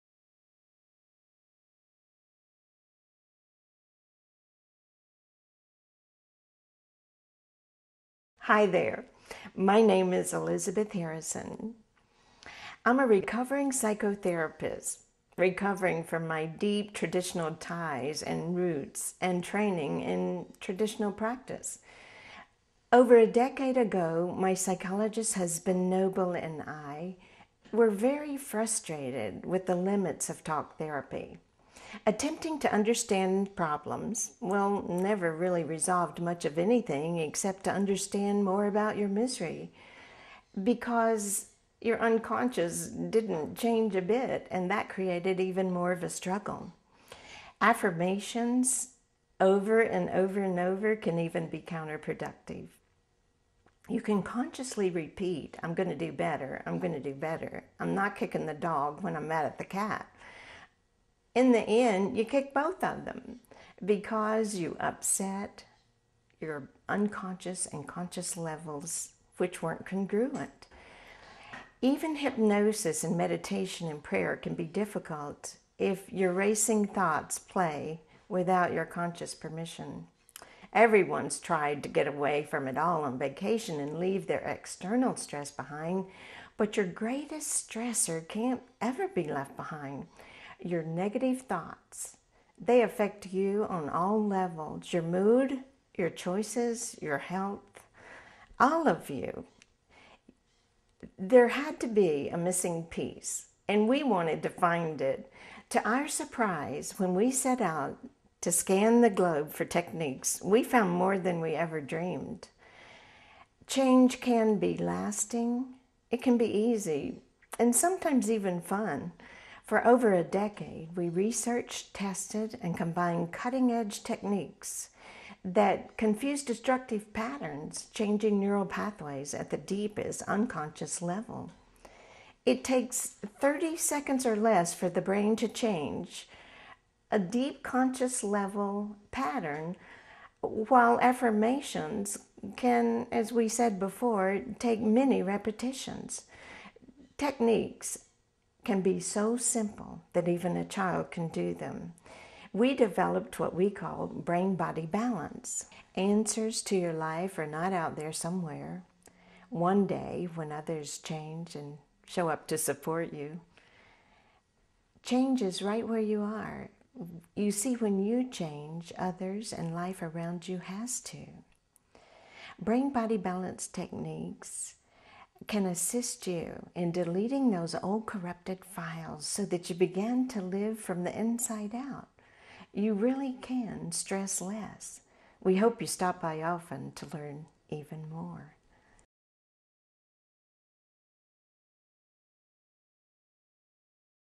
Brain Body Baance.mpg
For 15 years we combined, simplified & tested cutting edge energy techniques on 1000's of students with consistent, amazing results that defy traditional psychological practice limits with mood, relationships, prosperity and more!    Brain Body Balance™ Techniques take  minutes to learn and use for the change of your life!